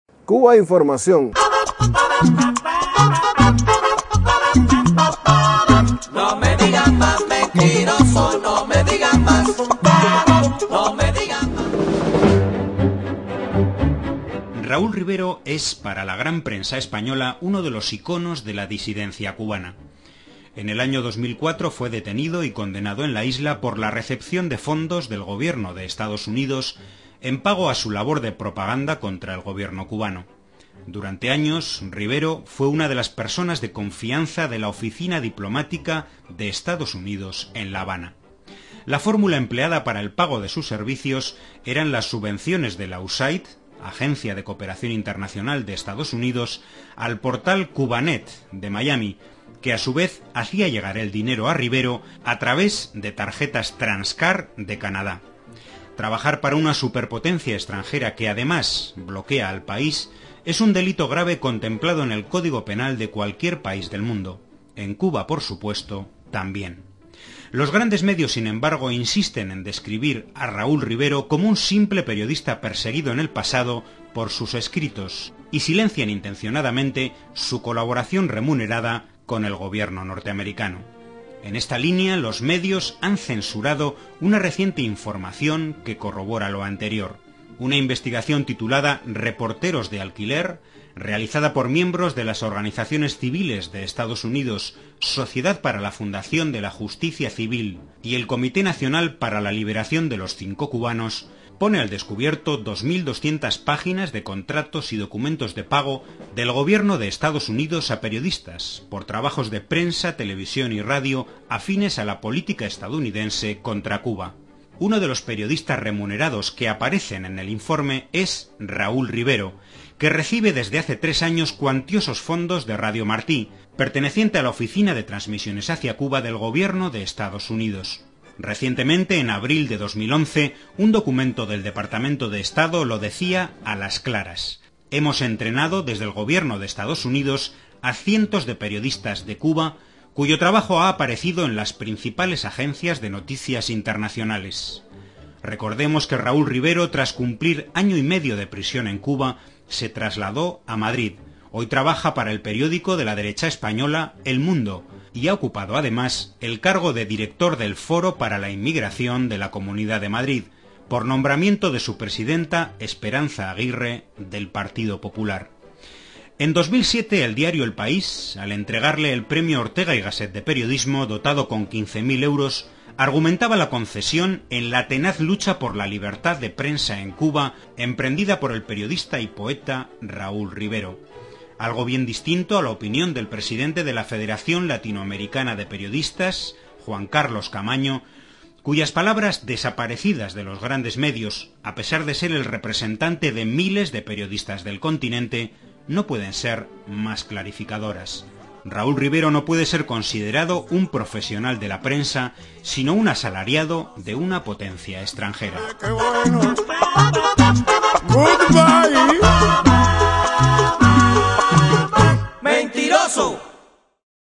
Raúl Rivero sigue cobrando del Gobierno de EEUU, igual que lo hacía en Cuba
La investigación "Reporters for Hire" (reporteros de alquiler) destapa 2.200 páginas de contratos y documentos de pago del Gobierno de EEUU a periodistas cubanos a su servicio, entre ellos el "disidente" Raúl Rivero.